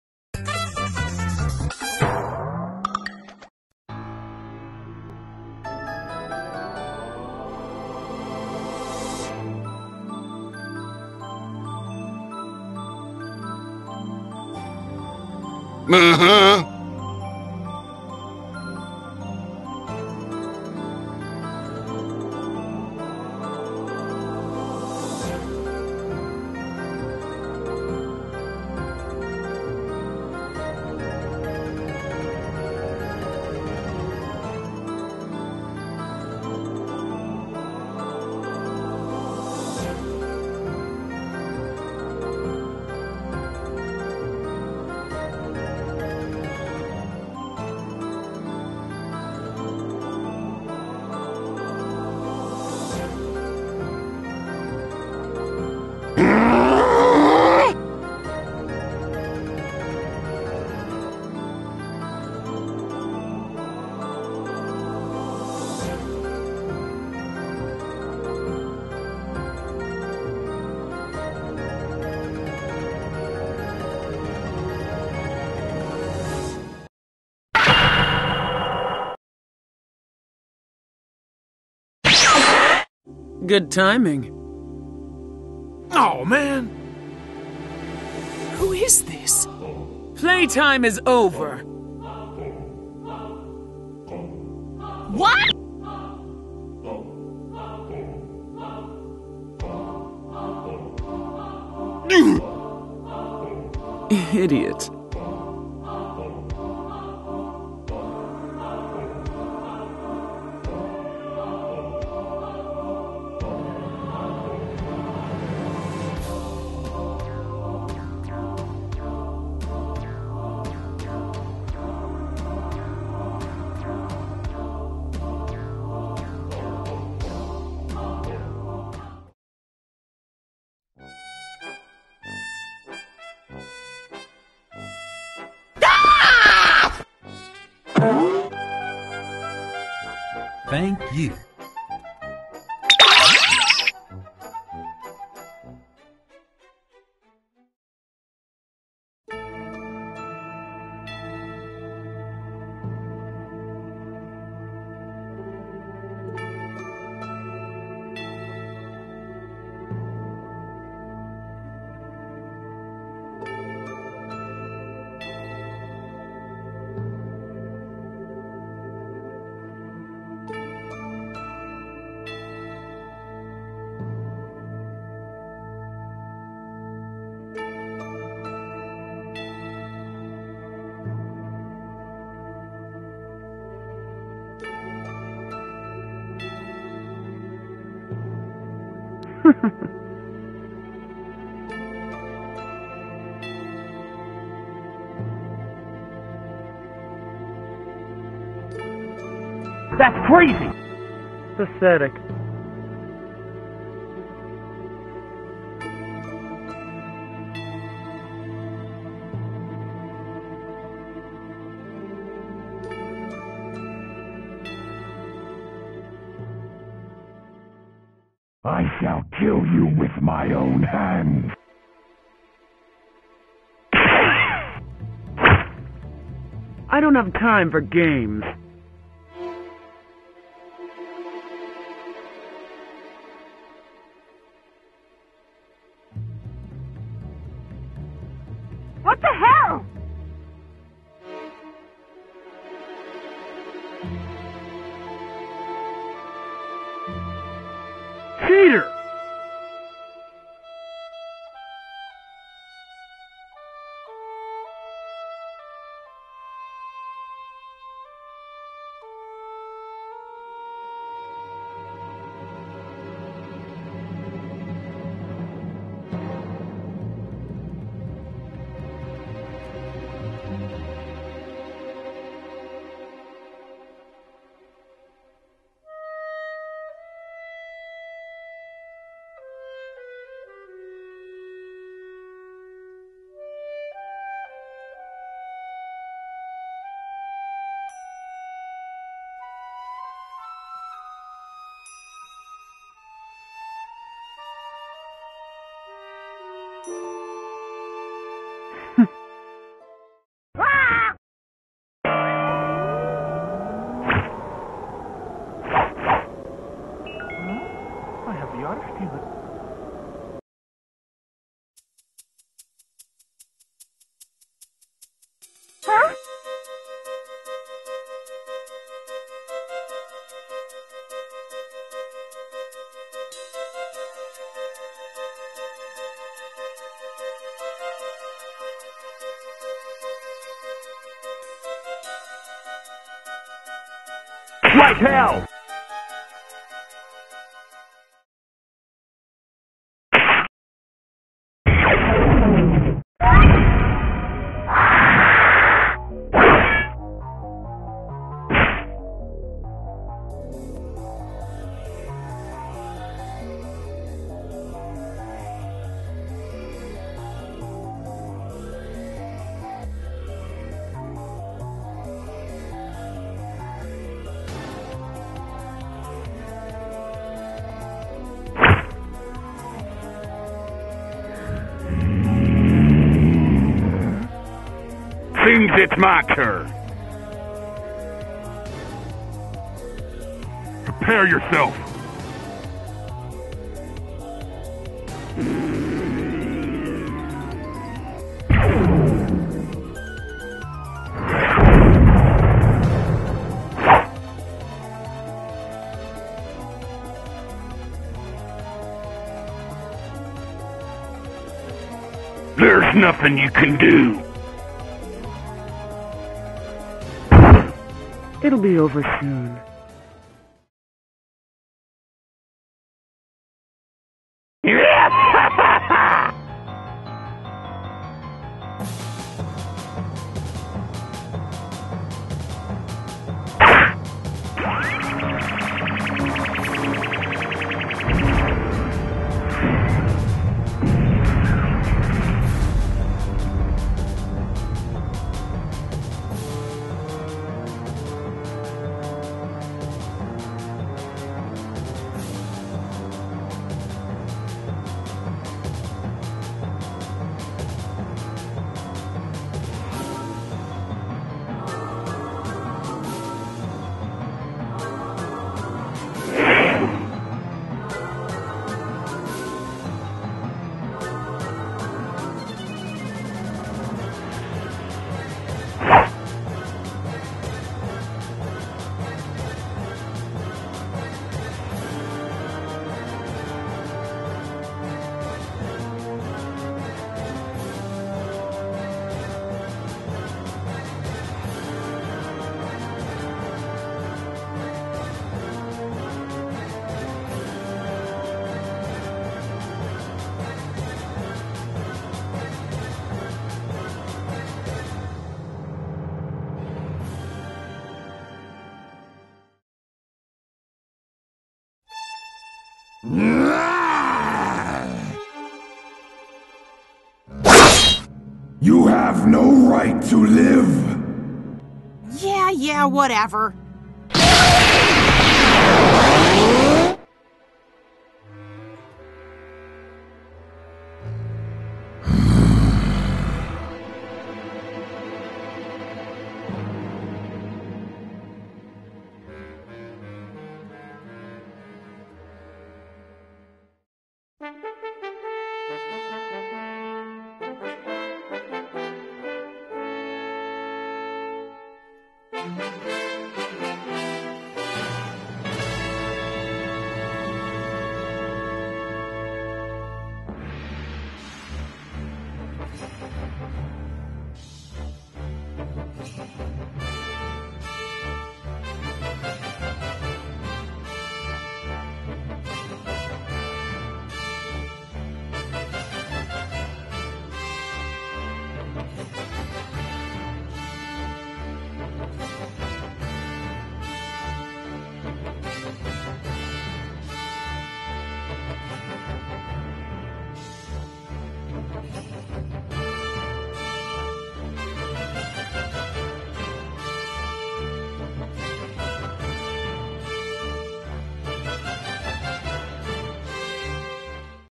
Slayers Unlimited Episode 13 part 2
Based on the novels made by Hajime Kanzaka and Rui Arazumi
and a fanfic made by frrobins (QueenHimiko)

The Scratch, Grounder and Coconuts sprites made by Tonberry2K

Opening
Vocals by Sapphire
Lyrics by Merr

and from now on I will be dividing the episodes into parts just to space the CPU of my new computer

lets say that it is a fan made episode
there might be some grammar errors here so go easy on me because English is not my main language, I am Portuguese